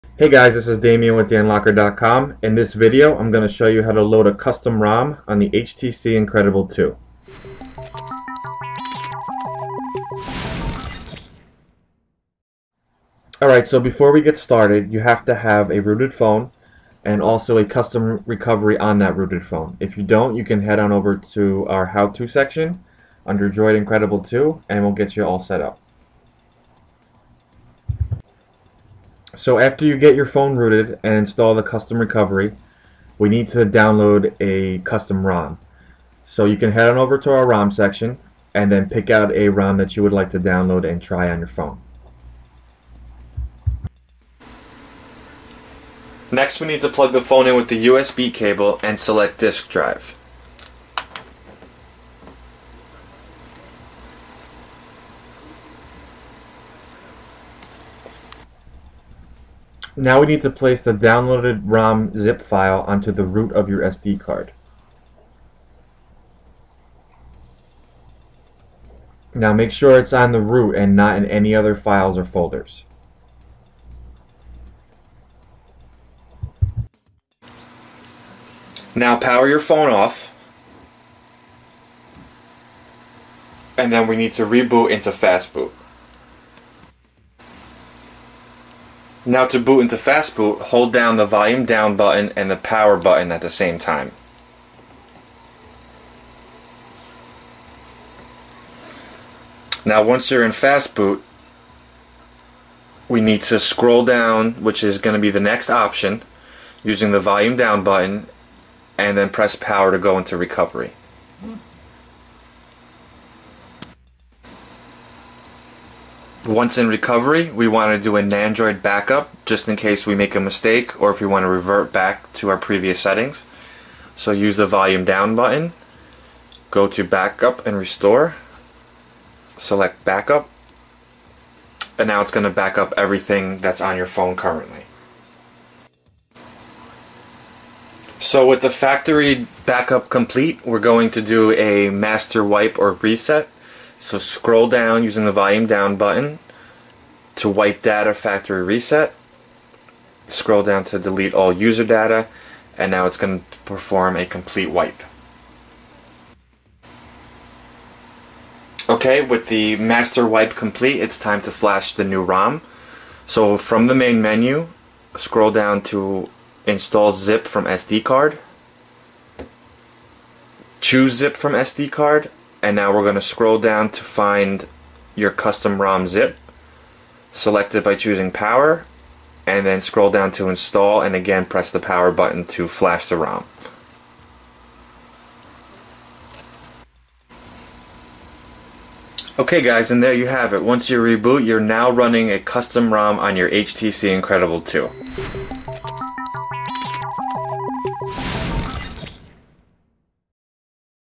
How To Load a Custom ROM on the Droid Incredible 2
For the necessary files or to see more of our how tos, head to the link above.

So you have rooted and loaded a custom recovery on your HTC Incredible 2. Now it's time for a custom ROM.